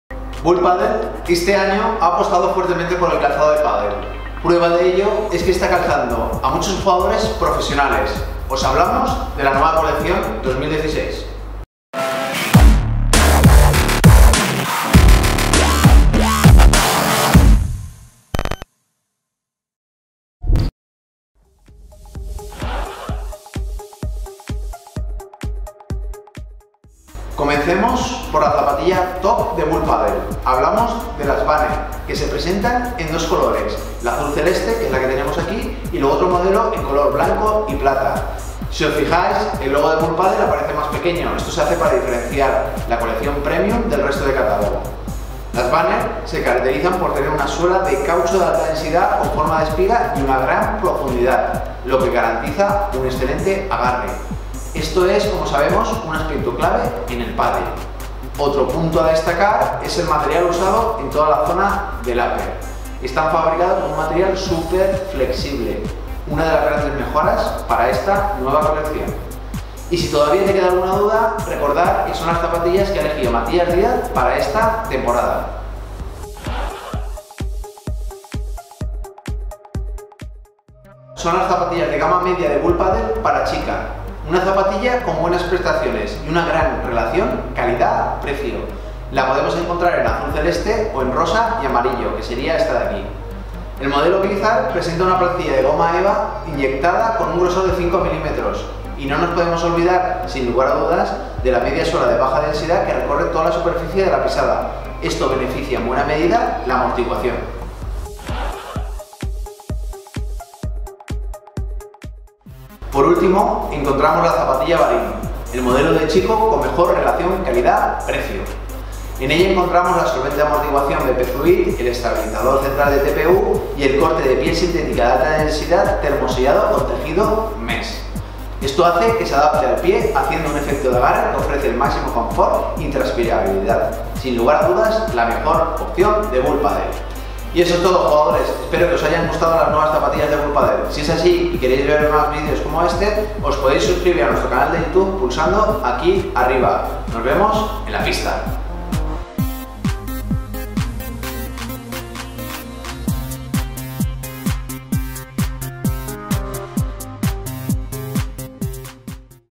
Review Zapatillas Padel Bullpadel 2016
Descubre las zapatillas de la marca Bullpadel, zapatillas, que están entre las de mejor calidad del mercado, responden perfectamente en cualquier terreno y situación, ofreciendo el máximo agarre en cada movimiento del jugador.

Las zapatillas de pádel Bullpadel comprenden muchos modelos destacados en cuanto a calzado del circuito profesional, hoy analizamos las 3 mejores zapatillas de Bullpadel.